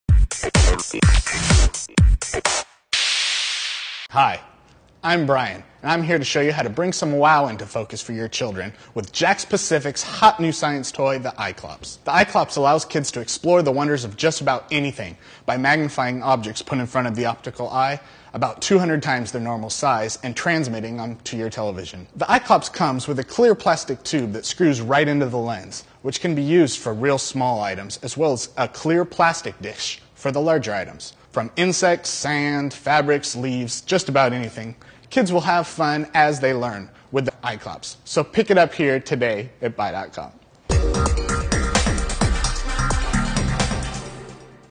BuyTV Spotlight Eyeclops
Get up close and personal with virtually any item! The Eyeclops is a handheld bionic microscope meant to be used as a fun toy - not as an educational aid. Simply plug it into any TV, turn it on and magnify any object up to 200 times its normal size. The image projects onto your TV. It''s a cinch to use as there are no complicated instructions to understand, software to install or system requirements necessary. Requires 5 "AA" batteries, not included.